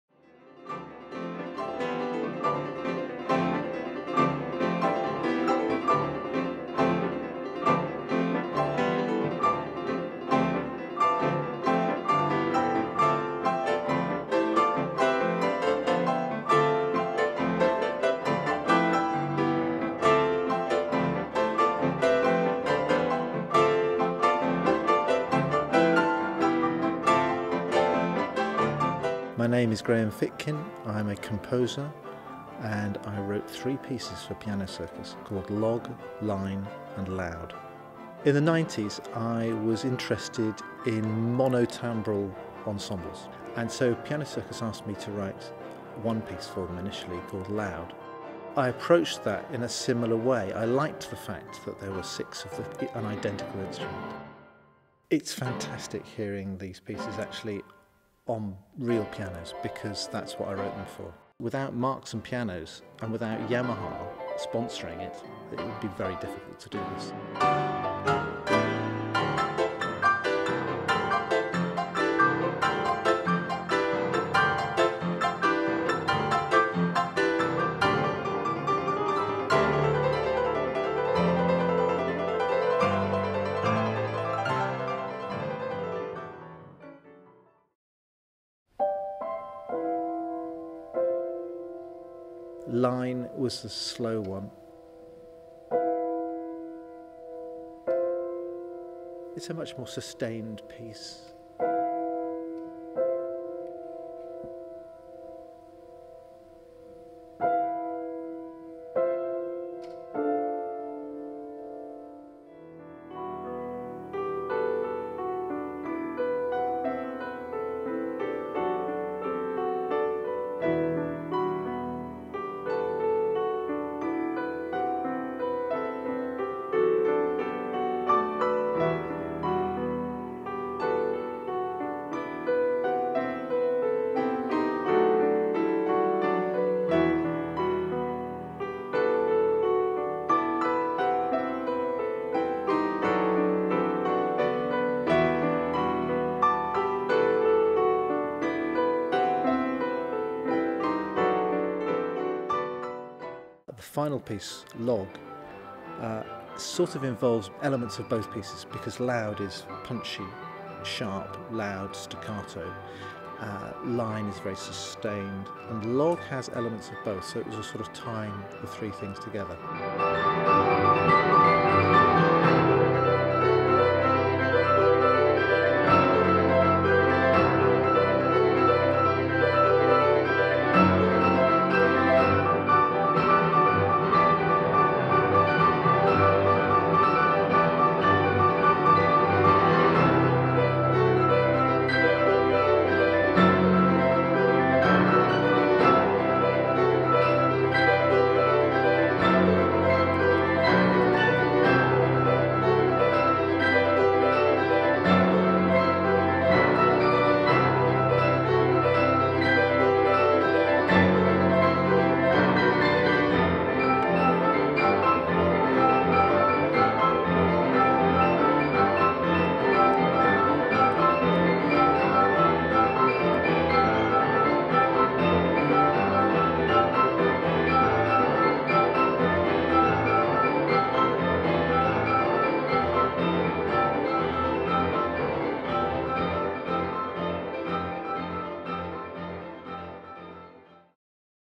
Piano Circus Graham Fitkin
Six of our Yamaha grand feature in a three-part series, shot on location at the Snape Maltings concert hall, UK.